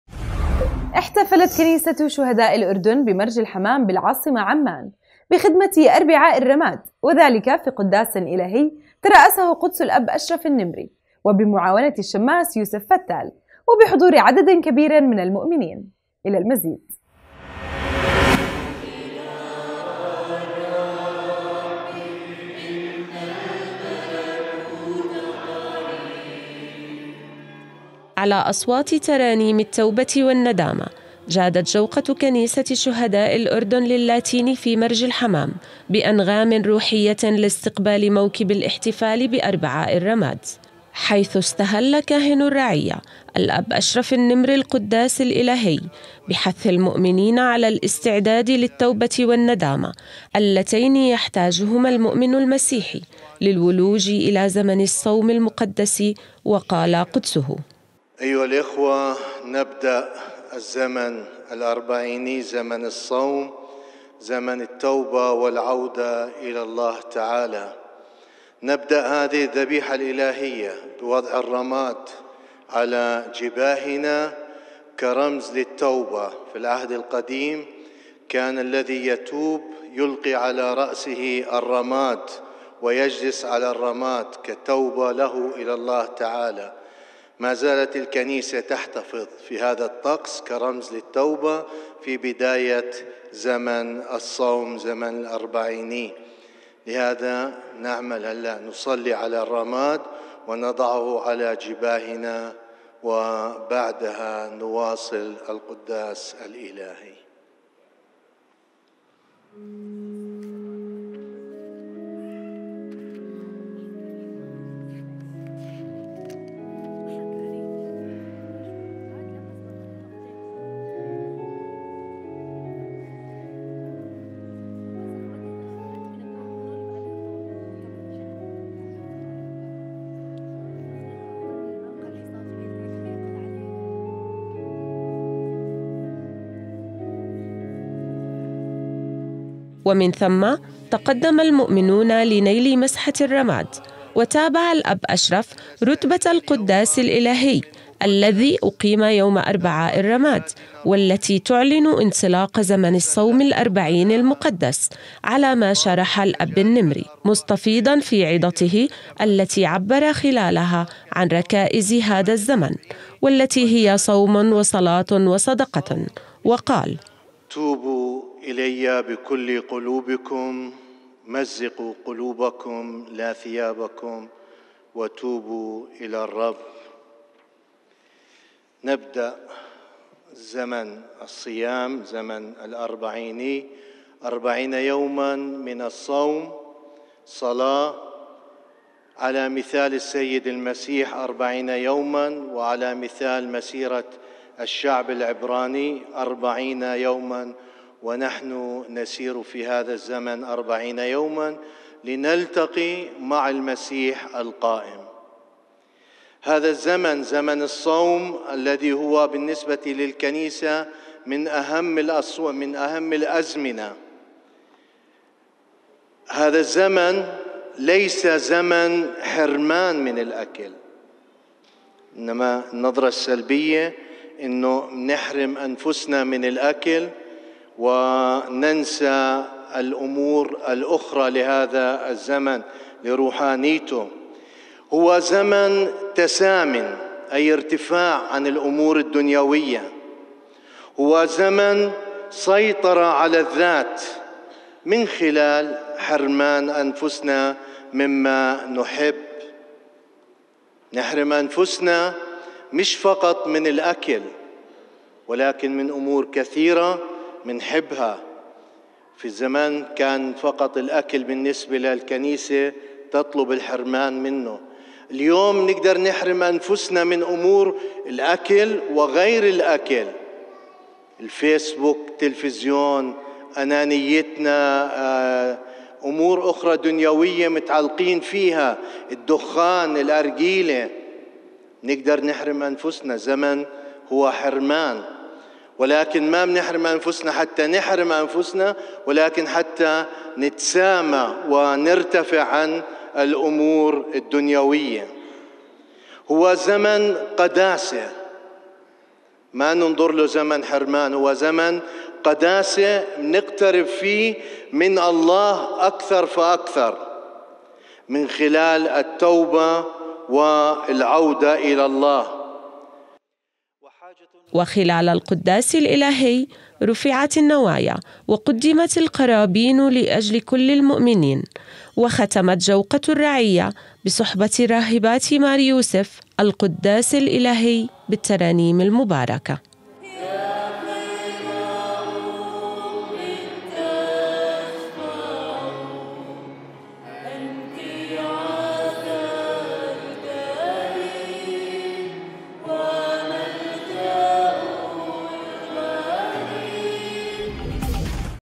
نورسات الأردن : كنيسةُ شهداءِ الاردن بمرجِ الحمام تحتفل بخدمةِ أربعاءِ الرماد
احتفلت كنيسةُ شهداءِ الاردن بمرجِ الحمام بالعاصمةِ عمان / بخدمةِ أربعاءِ الرماد / وذلك في قداسٍ الهي ترأسه قدسُ الأب أشرف النمري / وبمعاونةِ الشماس يوسف فتّال / وبحضورِ عددٍ كبيرٍ من المؤمنين .. الى المزيد .